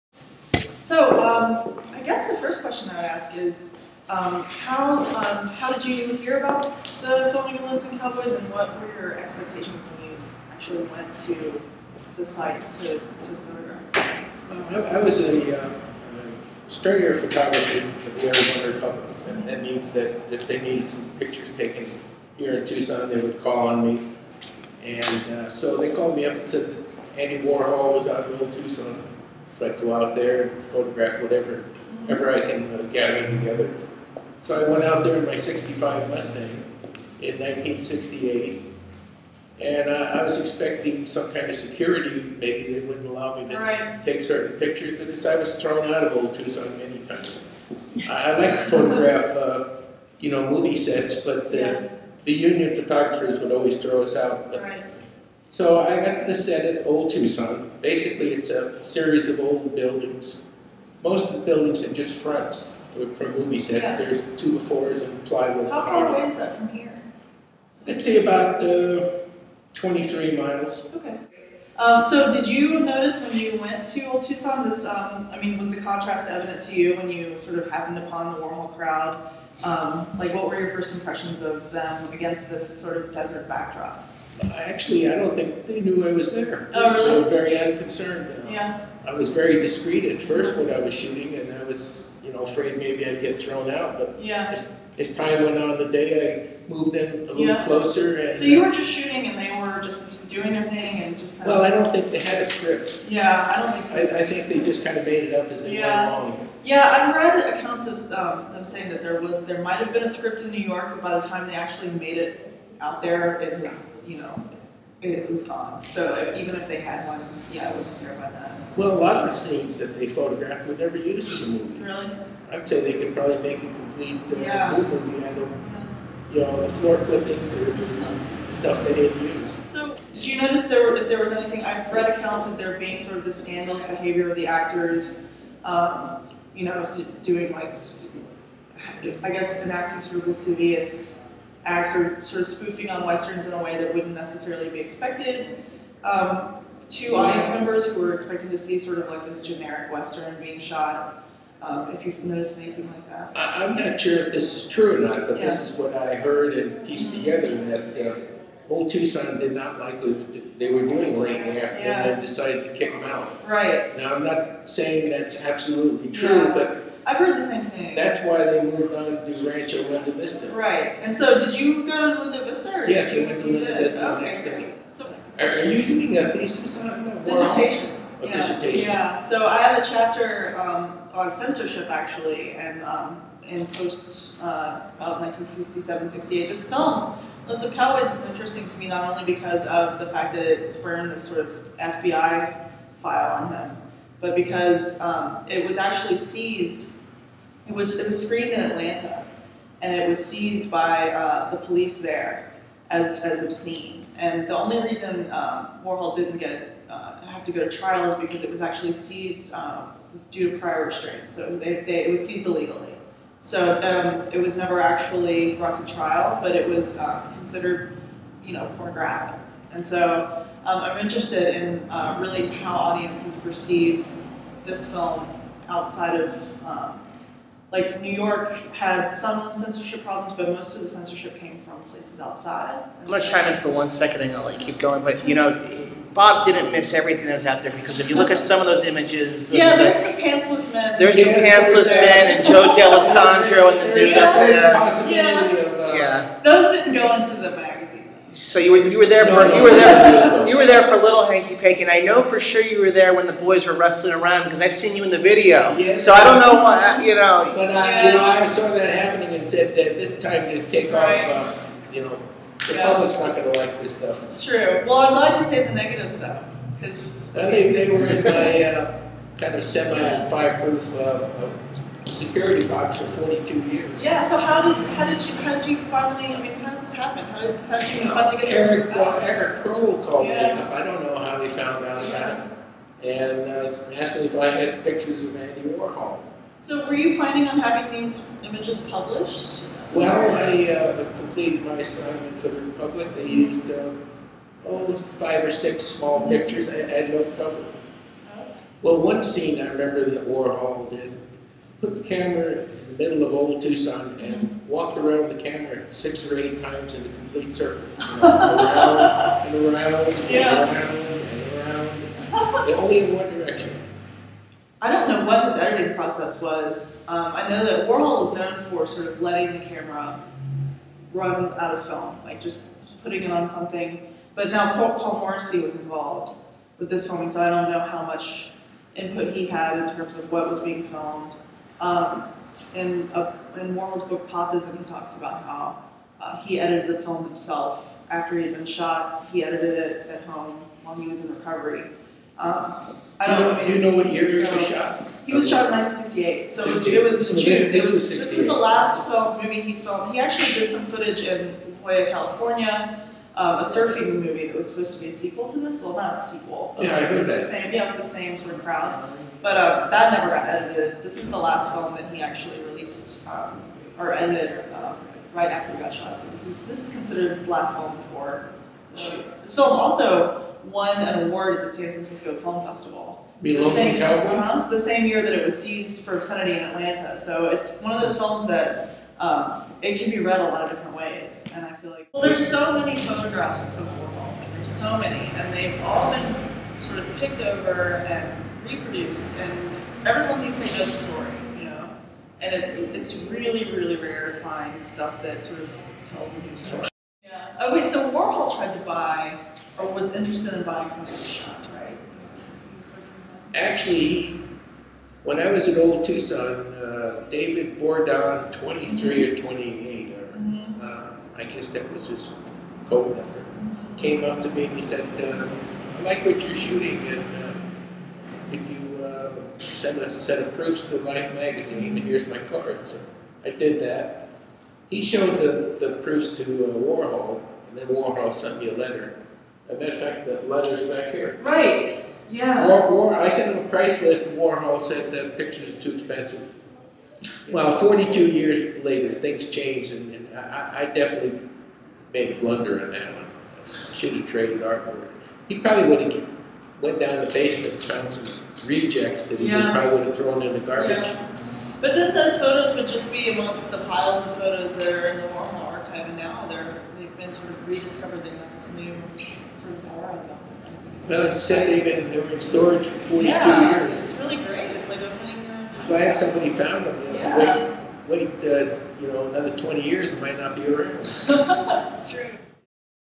Firestone Factory: Warhol Interviews (Chelsea Weathers and Broder 3/25/2010)
Interview with Chelsea Weathers and Bob Broder by Eric Firestone and. Shot March 25, 2010 at the "Warhol: Dylan to Duchamp" photo exhibition, Eric Firestone Downtown Gallery, Feb. 27-April 25, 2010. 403 N. 6th Ave.